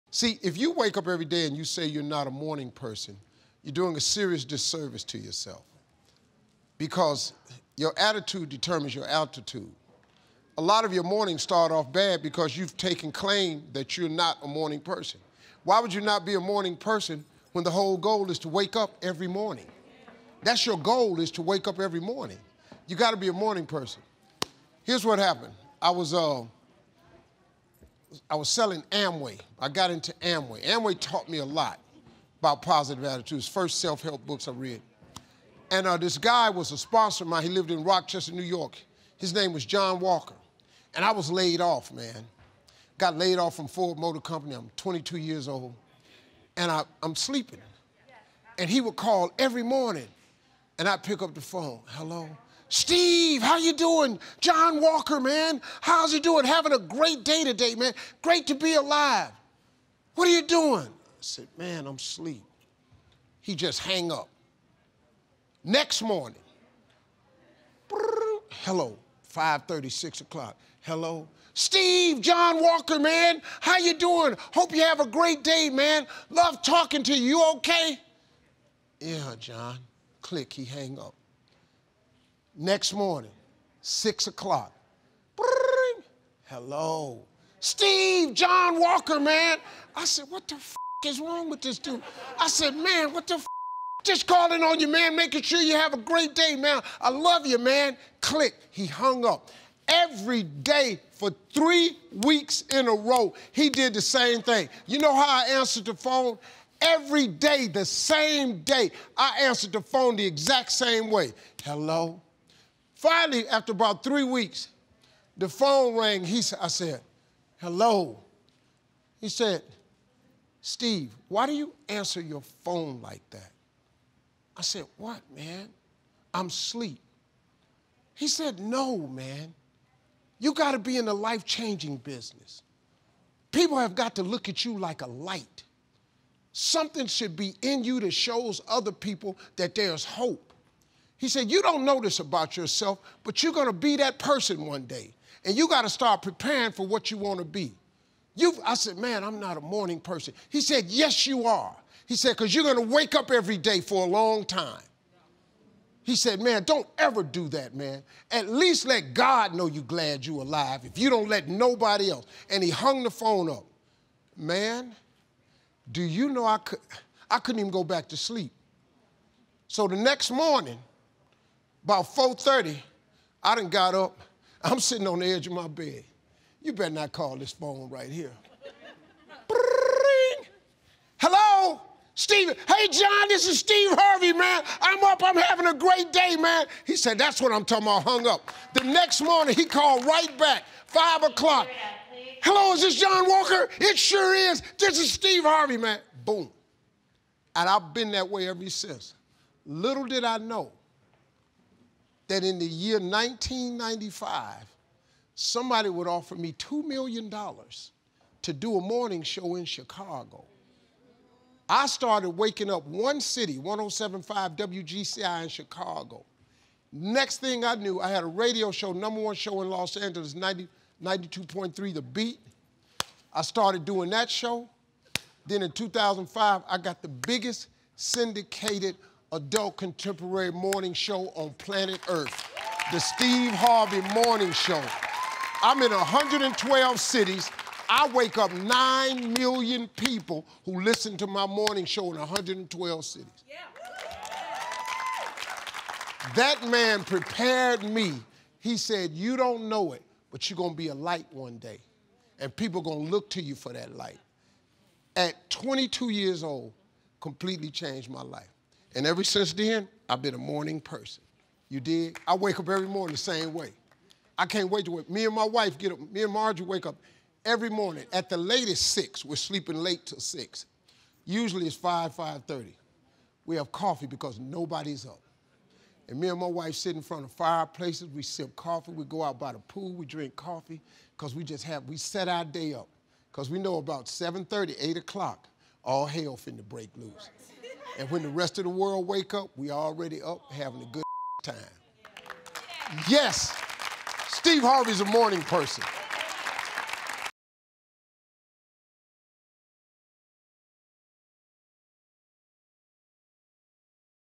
My Path To Radio | Motivational Talks With Steve Harvey
If you wake up every day and say you're not a morning person, you're doing a serious disservice to yourself. Because your attitude determines your altitude. I made a change and it changed my life.